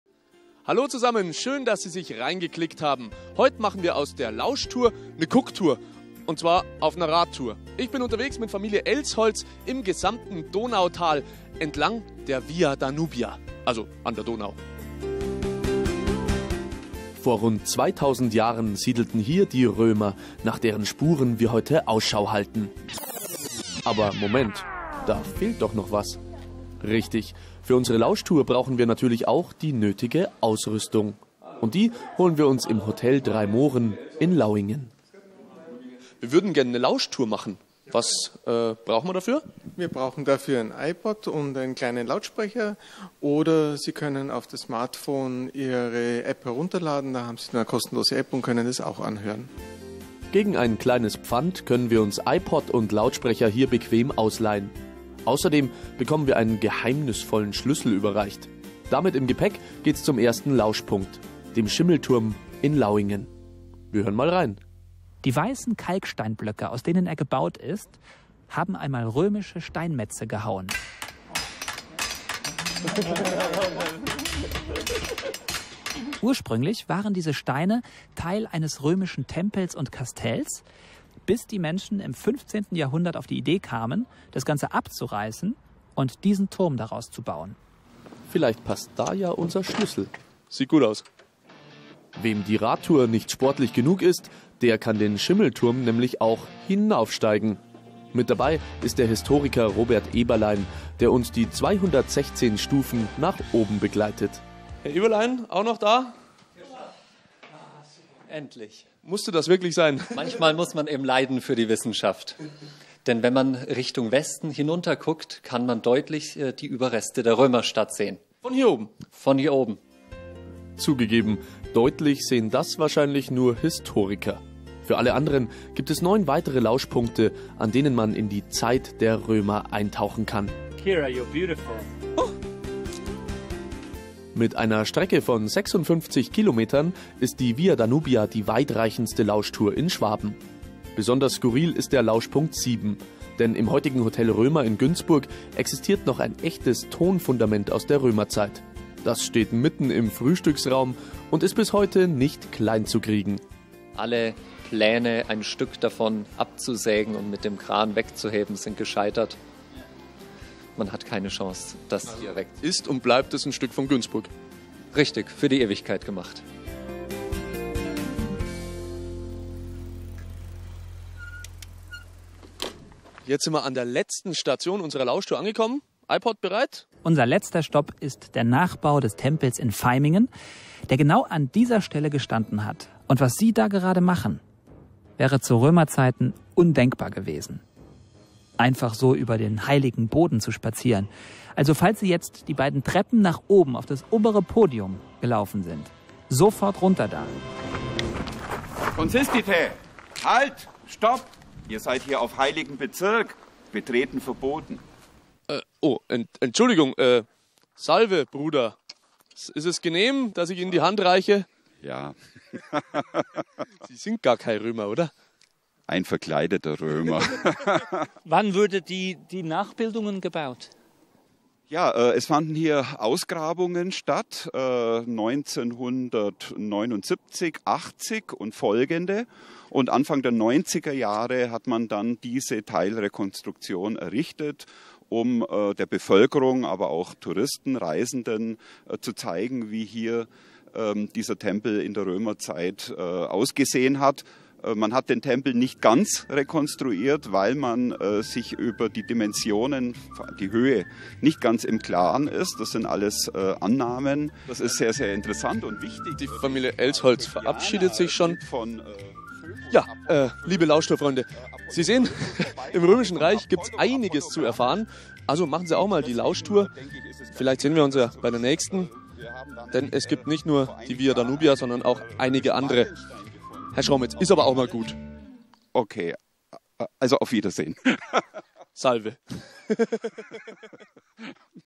Radeln auf Römerspuren im Schwäbischen Donautal - Lauschtour Via Danubia
Sogar Geschichtsmuffel werden Augen machen! Diese Radtour versetzt Sie zurück in die Zeit der Römer. Halten Sie als Wachsoldat auf einem Turm nach wilden Germanen Ausschau und entspannen Sie hinterher am Tempel. In den 400 Jahren, in denen die Römer hier in der Provinz Rätien waren, haben Sie viele überraschende Spuren hinterlassen! Also radeln Sie mit über die "Via Danubia", eine berühmte Römerstraße, die durch das wunderschöne Donautal führt.